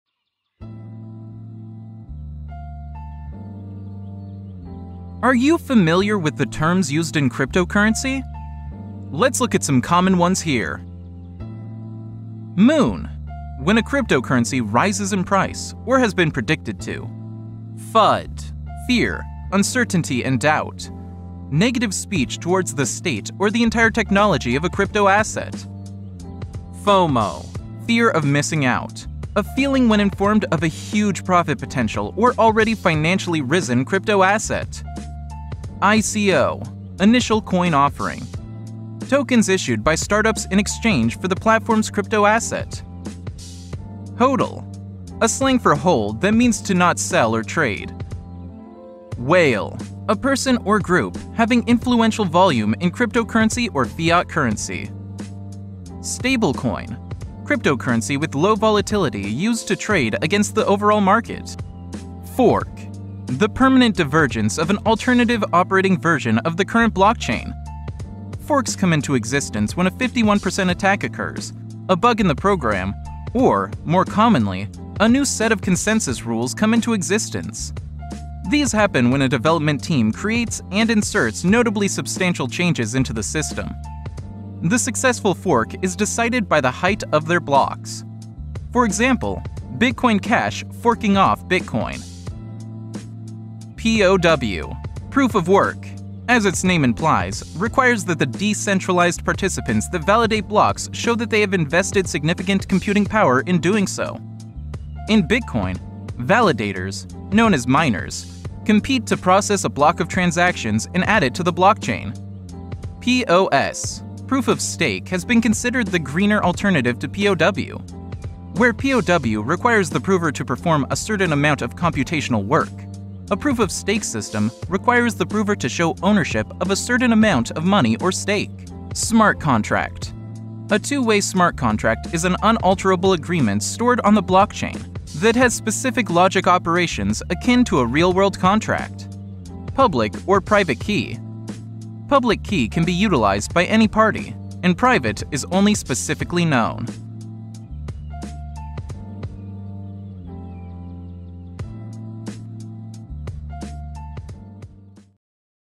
Cryptocurrency - The Lingo
This video explains some of the most common terms used in cryptocurrency.